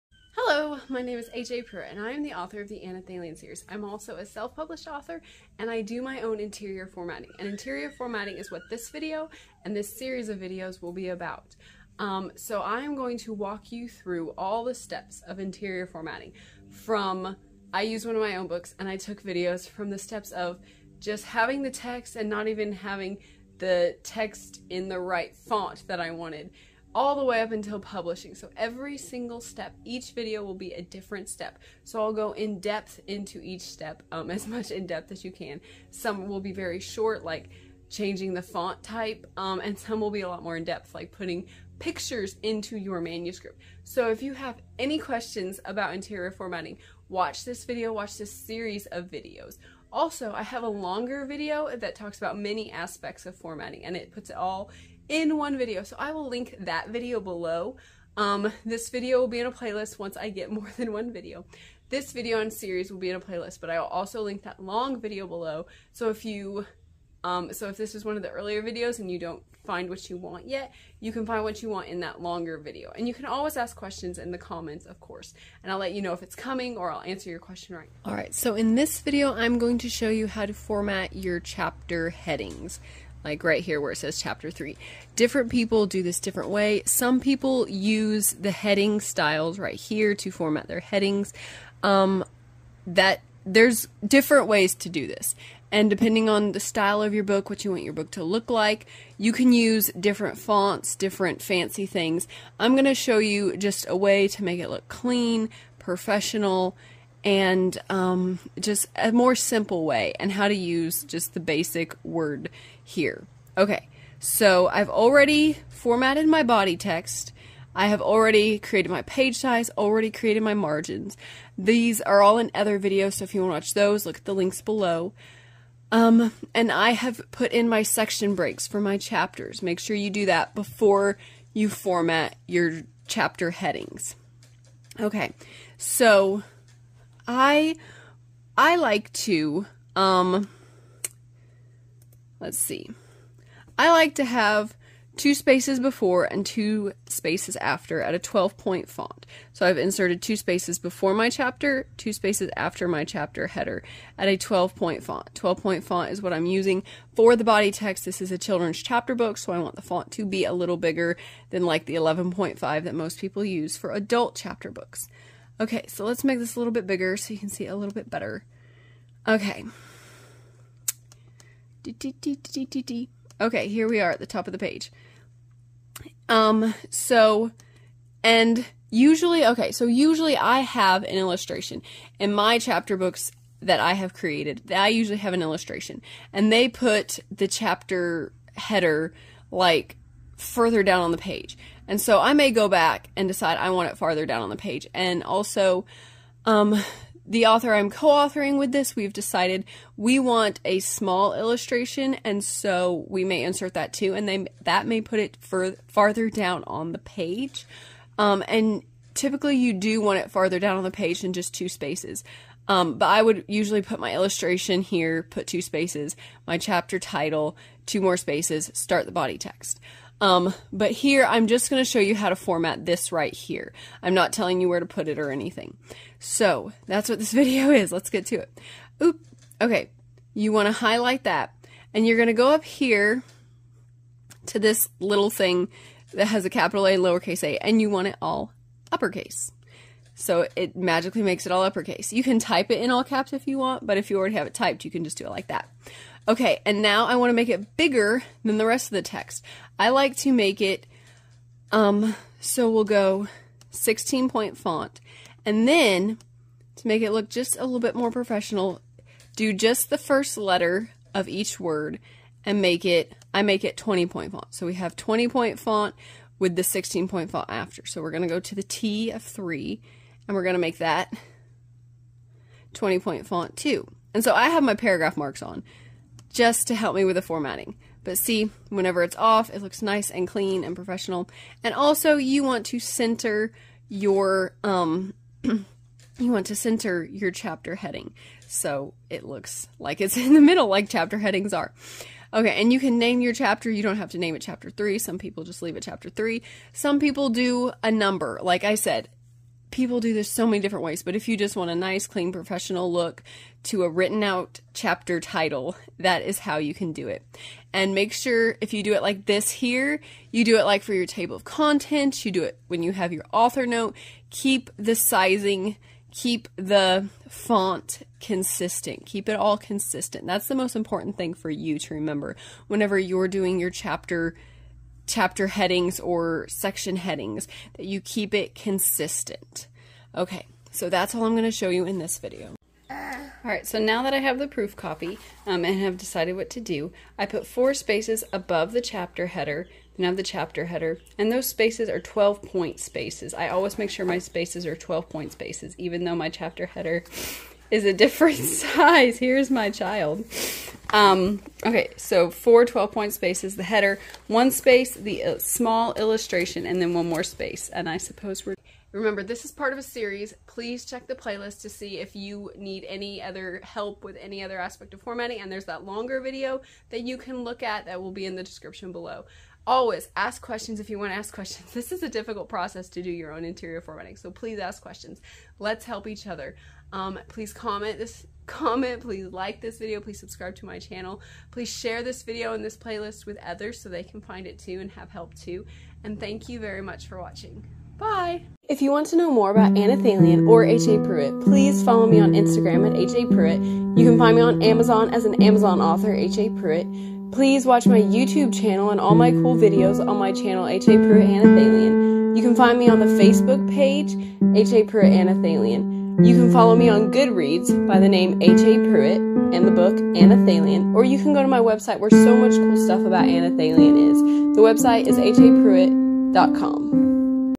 How To FORMAT A BOOK In Word • Video 5: Chapter Headers
This video is part of a series about how to format your book using Word. This series describes how authors can create the interior formatting for a book they plan to self-publish. This series could also provide helpful tips for anyone trying to create an essay for school, professional dissertation, or document that requires specific formatting.

This is the third video in the interior formatting series by H.A. Pruitt.

This video describes how to create chapter headings. Once I inserted the small chapter illustration (separate video), I chose to insert four spaces above the chapter heading.

Make sure to do the other steps in this playlist before moving to this step.

To see a longer video that briefly discusses many aspects of interior formatting, follow this link: